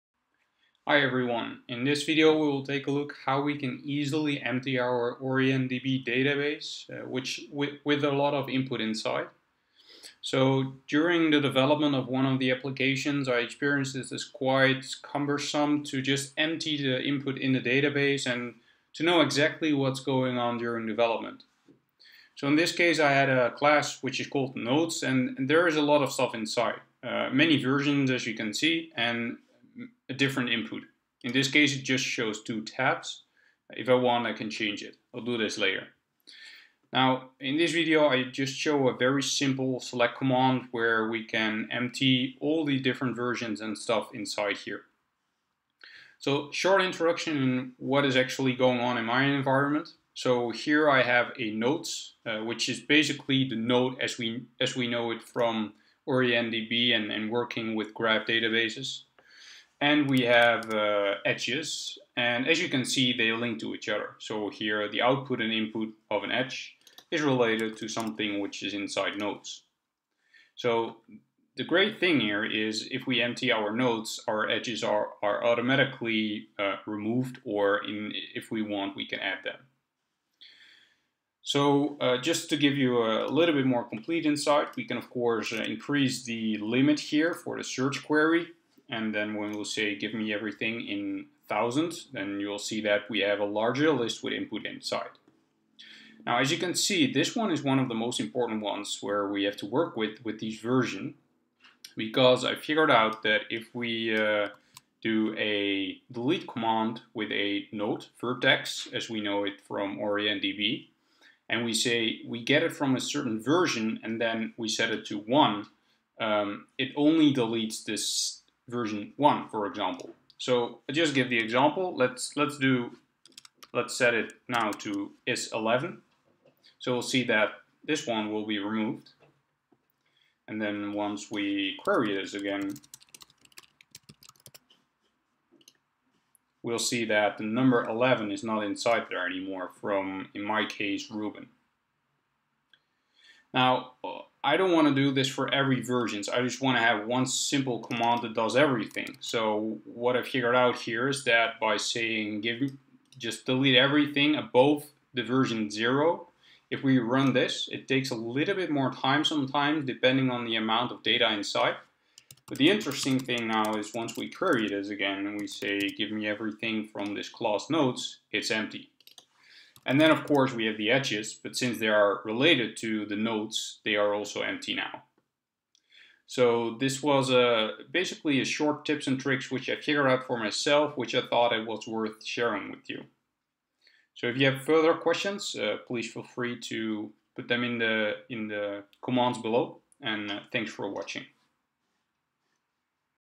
OrientDB content vertex or edge class
OrientDB remove content in vertex or edge class easily